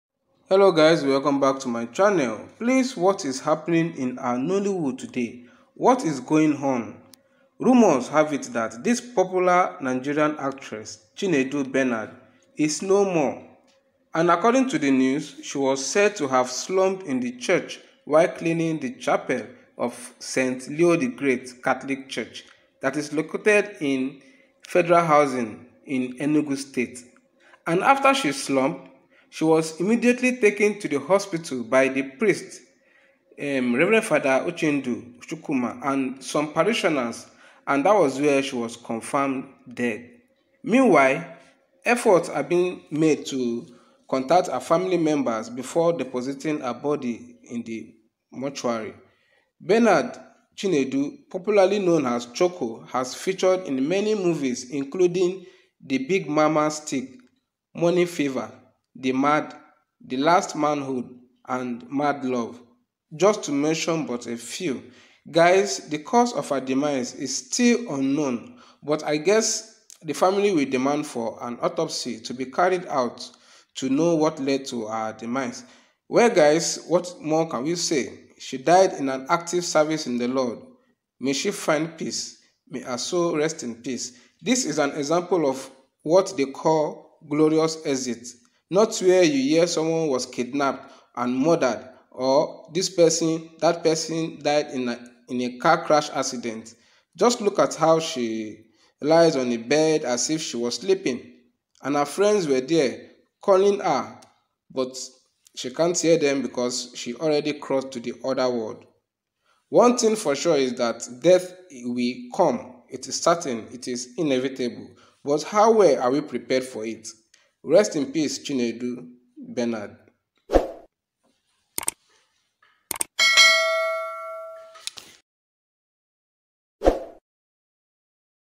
😭😭Not Again Nollywood Actress Chinedu Bernard Slumps
Delectable and beautiful Nollywood actress Chinedu Bernard was confirmed d*ad at the East Side hospital Enugu after she slumped while cleaning the Chapel of St. Leo the Great Catholic Church Federal Housing.

The Priest Rev Fr. Uchendu Chukwuma and some parishioners rushed her to the hospital where she was declared d*ad.